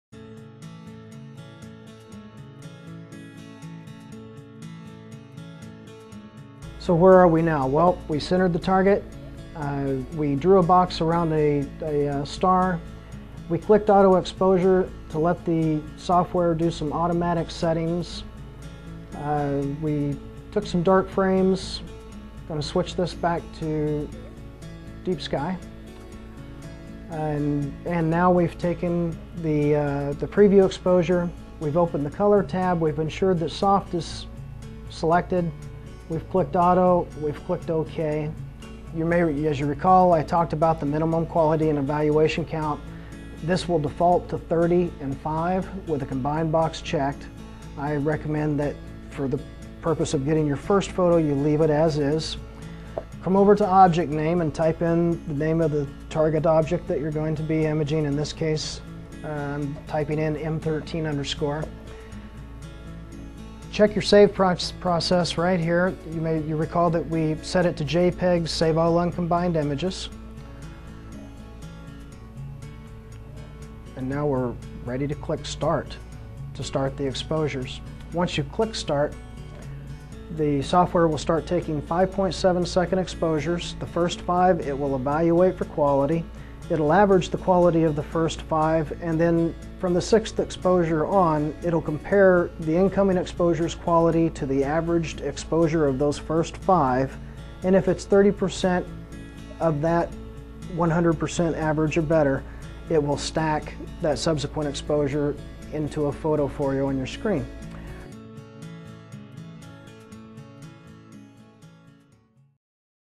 Meade DSI Envisage Tutorial - Part 7
How to capture your images in the Envisage software.